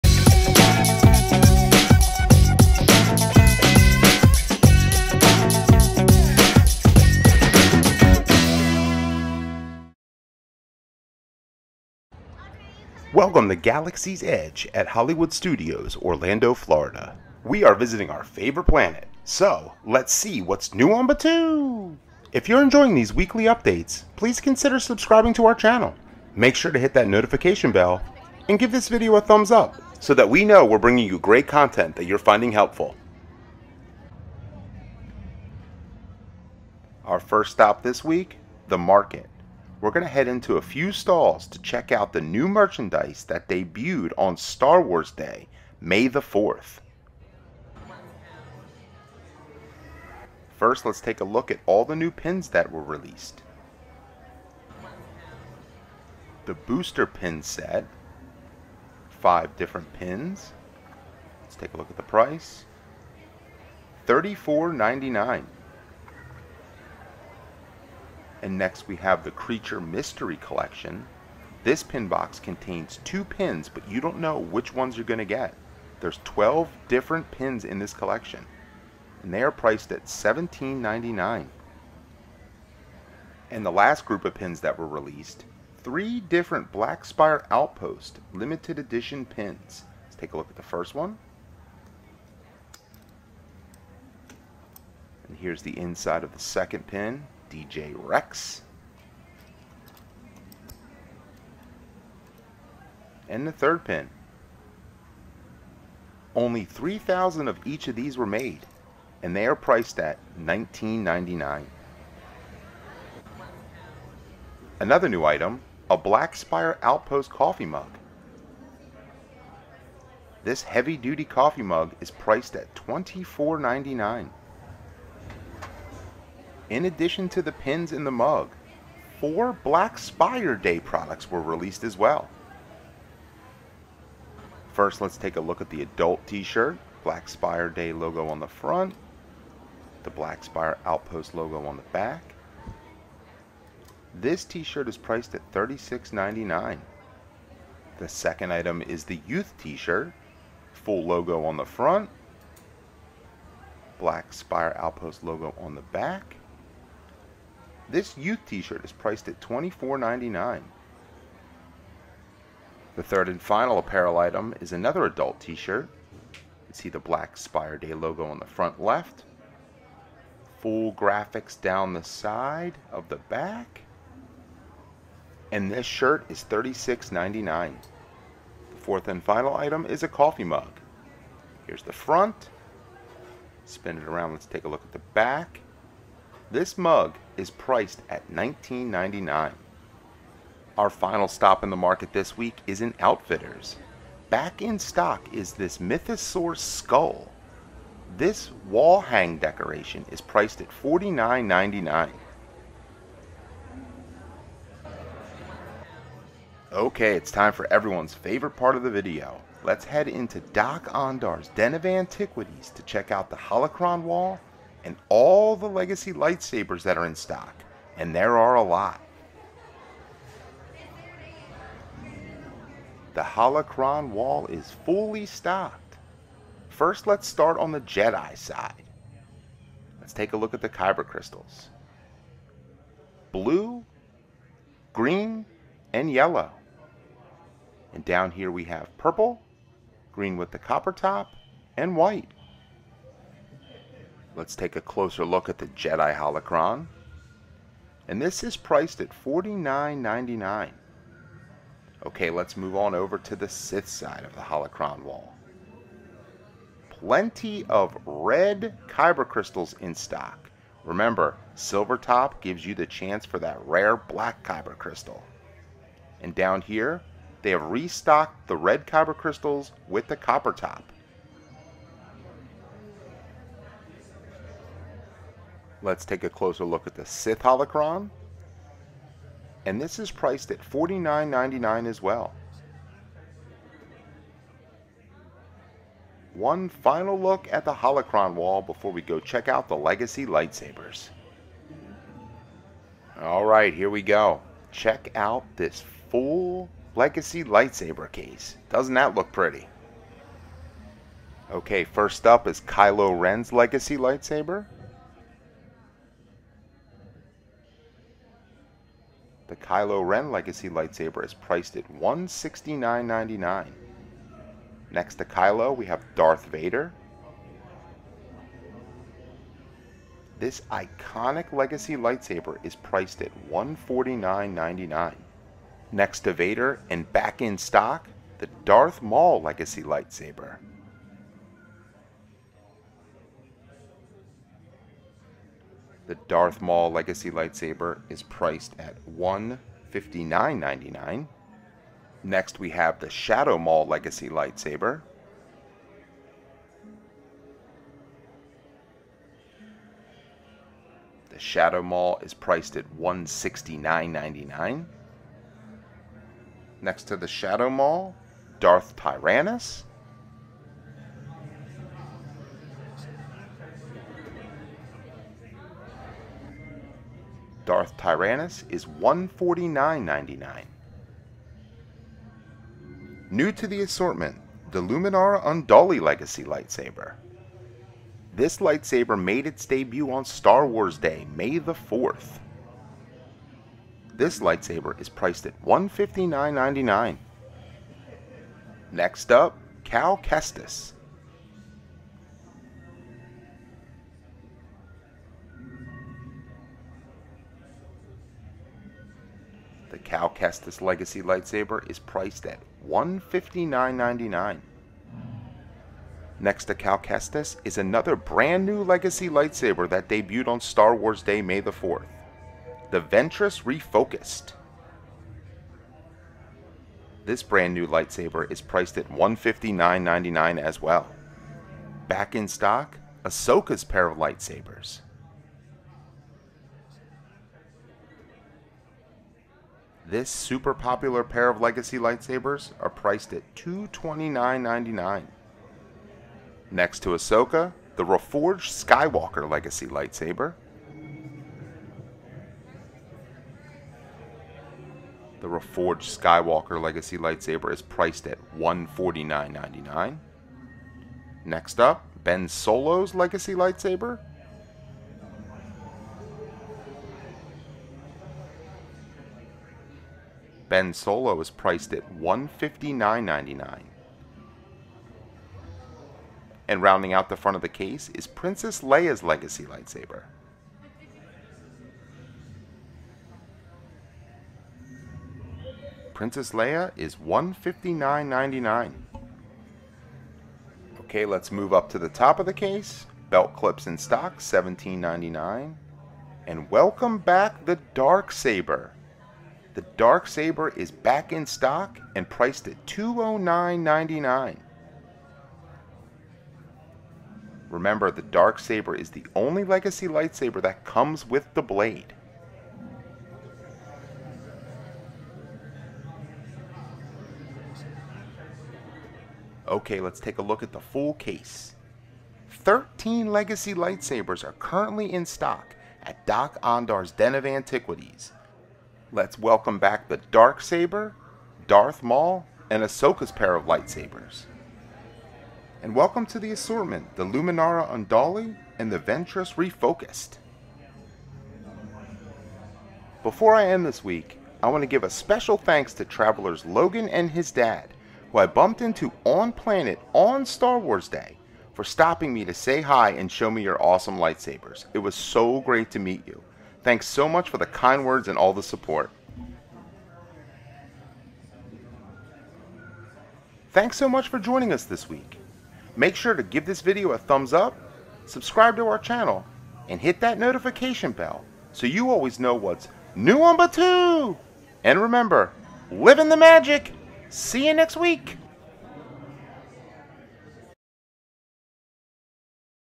Legacy Lightsaber Stock Update HUGE RESTOCK | Star Wars Galaxy's Edge Hollywood Studios | Disney 4K
May 8th, 2022 -  We are back at Hollywood Studios and on planet Batuu! Join us for our weekly Legacy Lightsaber update! What's new on Batuu! This week we show EVERY lightsaber available at Doc-Ondar's Den of Antiquities at Disney's Hollywood Studios. Huge restock plus 2 new lightsabers arrived! We also take a look at the Kyber Crystal wall!